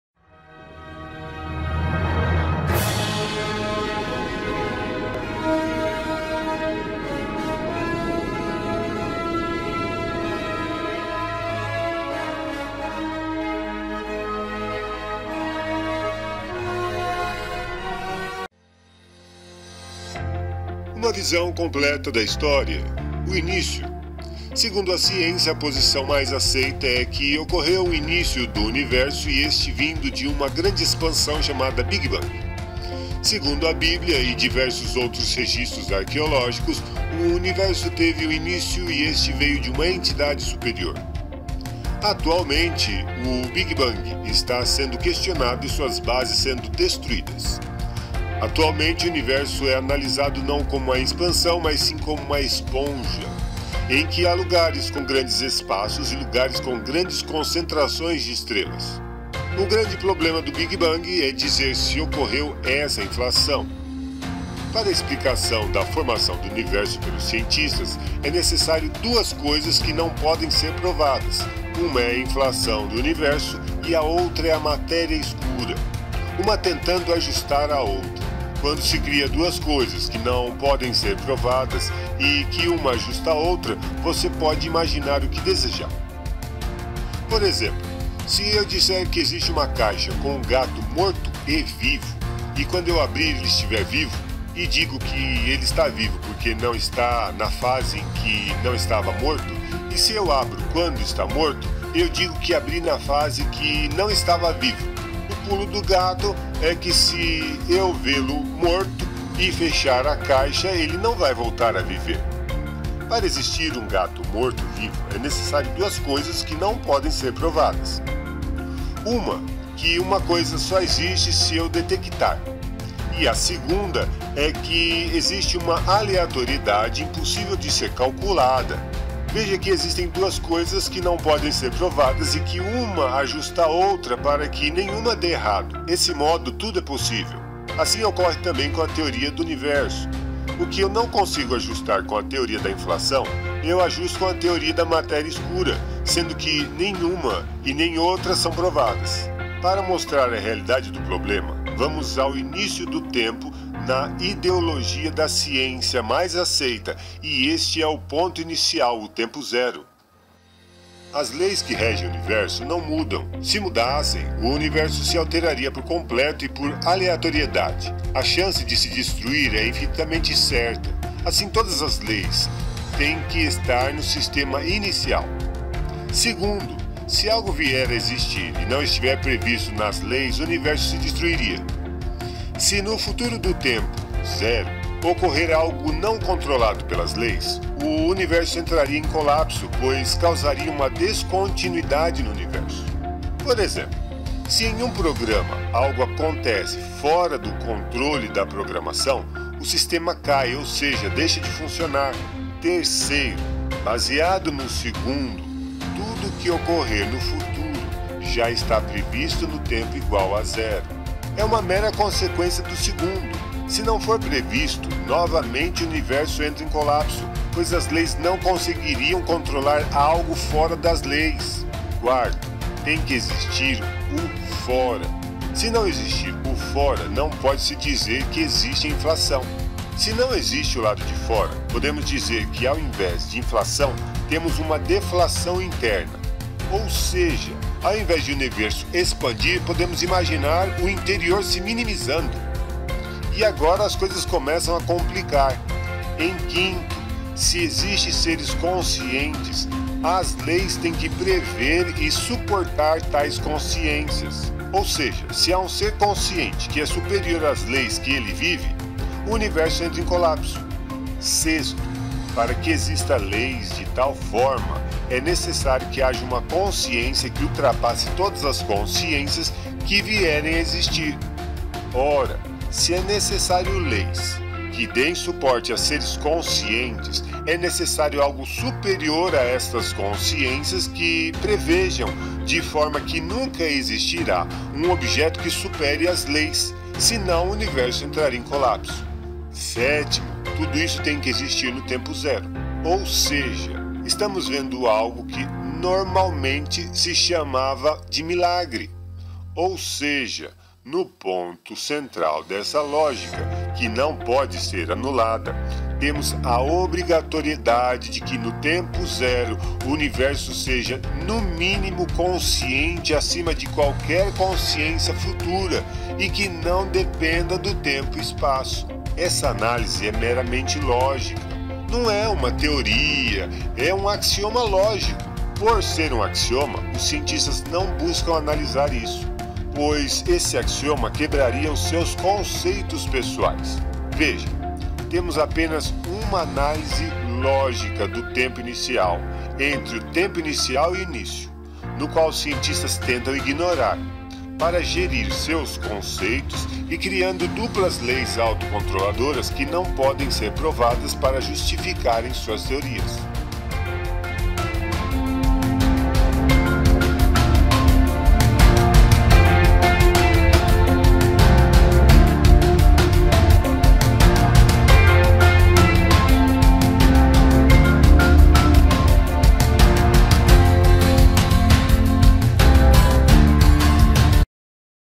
Inflação Universal - Ocorreu? - VC1-E4-P
Segundo a ciência a posição mais aceita é que ocorreu um início do universo e este vindo de uma grande expansão chamada Big Bang.
Segundo a bíblia e diversos outros registros arqueológicos, o universo teve um início e este veio de uma entidade superior.
Atualmente, o Big Bang está sendo questionado e suas bases sendo destruídas. Atualmente o universo é analisado não como uma expansão mas sim como uma esponja.
Em que há lugares com grandes espaços e lugares com grande concentração de estrelas.
O grande problema do Big Bang é dizer se ocorreu essa inflação. Para explicação da formação do universo pelos cientistas é necessário duas coisas que não podem ser provadas, uma é a inflação do universo e a outra é a matéria escura. Uma tentando ajustar a outra, quando se cria duas coisas que não podem ser provadas e que uma ajustar a outra. Você pode imaginar o que desejar!
Por exemplo: se eu disser que existe uma caixa com um gato morto e vivo, e quando eu abrir ele estiver vivo e digo que ele está vivo pois não está na fase em que não estava morto. E se eu abro quando está morto, eu digo que eu abri na fase que não estava vivo. O pulo do gato é que se eu vê-lo morto e fechar a caixa ele não vai voltar a viver.
Para existir um gato morto-vivo é necessário duas coisas que não podem ser provadas, uma que uma coisa só existe se eu detectar e a segunda é que existe uma aleatoriedade impossível de ser calculada.
Veja que existem duas coisas que não podem ser provadas e que uma ajustar a outra, para que nenhuma dê errado. Esse modo tudo é possível.